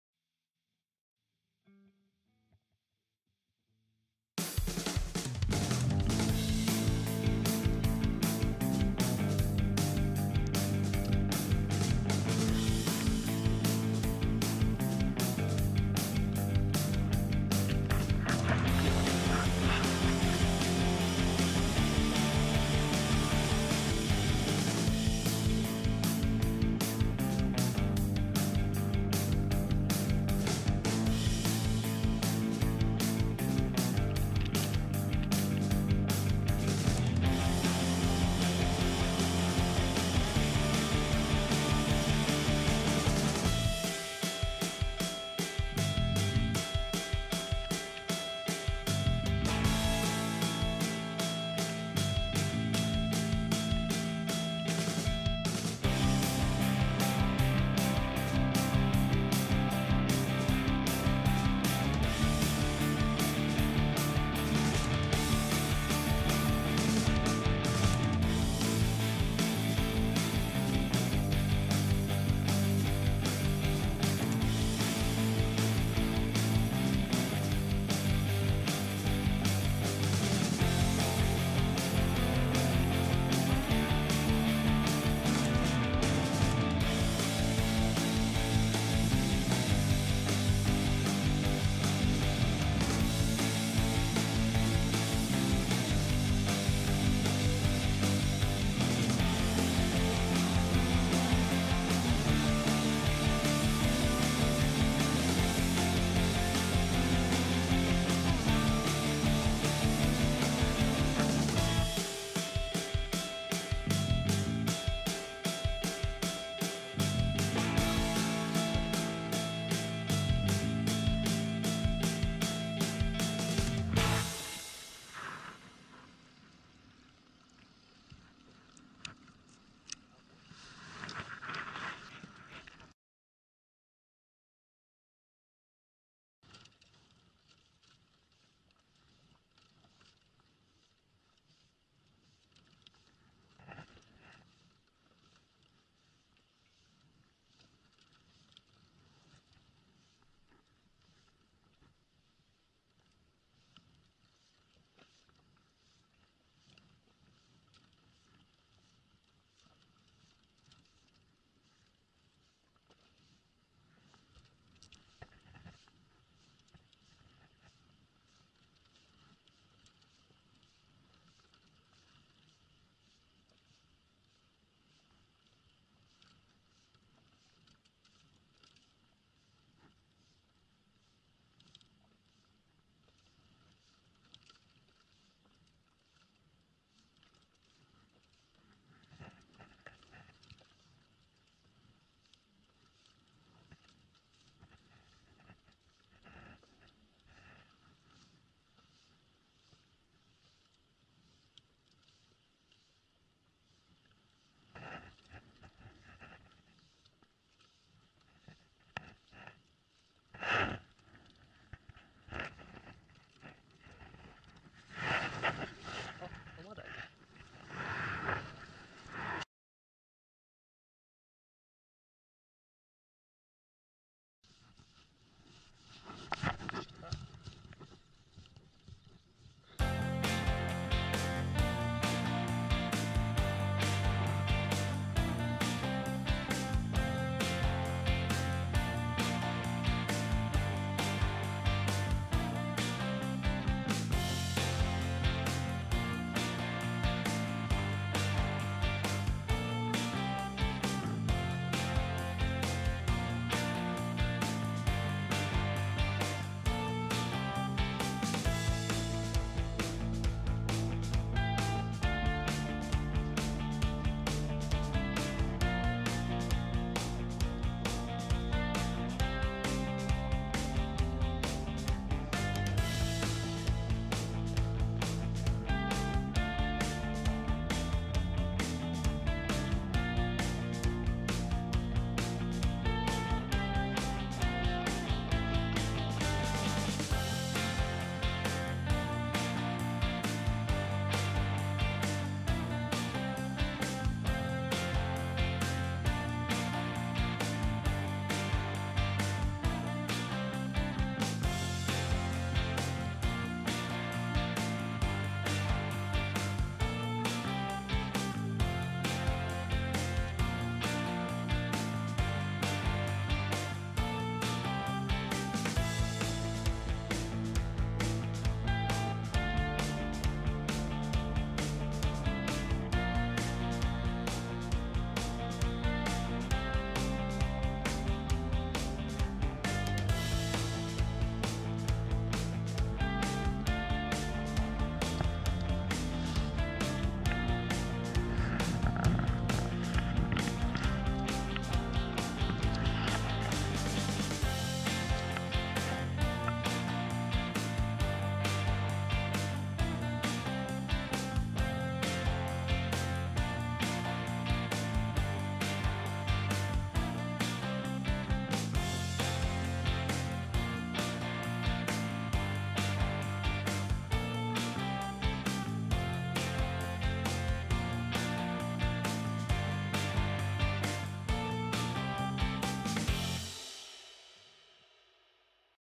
ゴムボート釣りで春の鯛狙い、釣れたのは鯛は鯛でも・・・ Tilefish/Deep-sea porgy
ゴムボートで春の桜鯛狙いで出港。根魚中心にいろいろ釣れました。鯛と名のつく魚も釣れましたが・・・